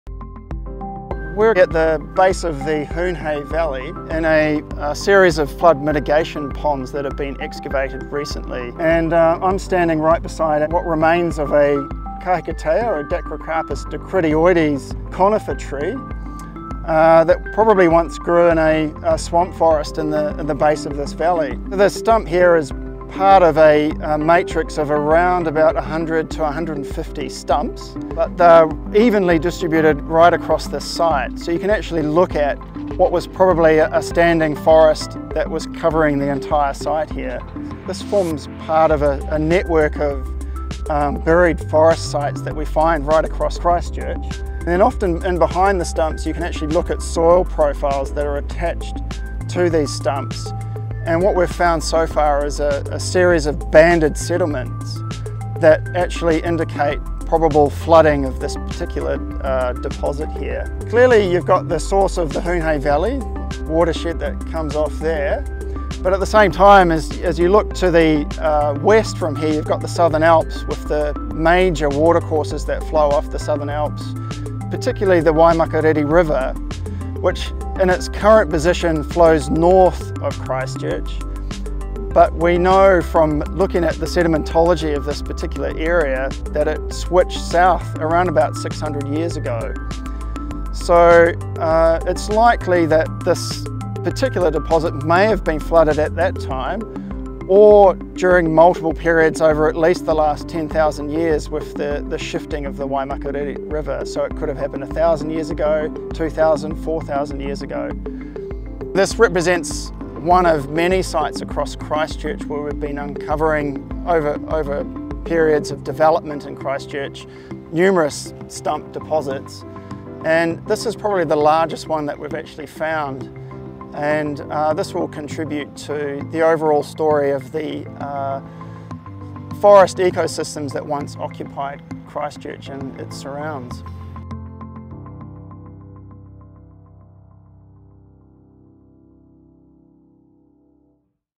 Ancient tree stumps uncovered
University of Canterbury fossil plant specialist Dr Matiu Prebble talks about the discovery of tree stumps from an ancient forest in the Hoon Hay Valley in Christchurch